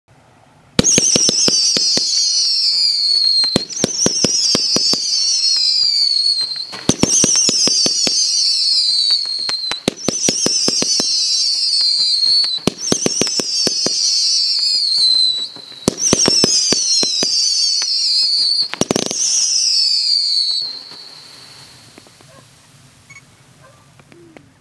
Falling Leaves by Planet Fireworks
Falling Leaves cake by Planet Fireworks (made by Broekhoff). Filmed by UKFR.COM